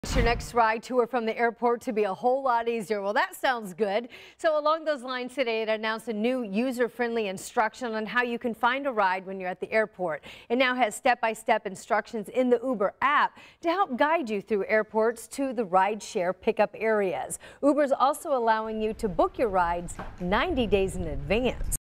Uber's new features to help airport pickup
Uber wants your next ride to or from the airport to be easier. Today, it announced new user friendly instructions on how to find your ride at airports.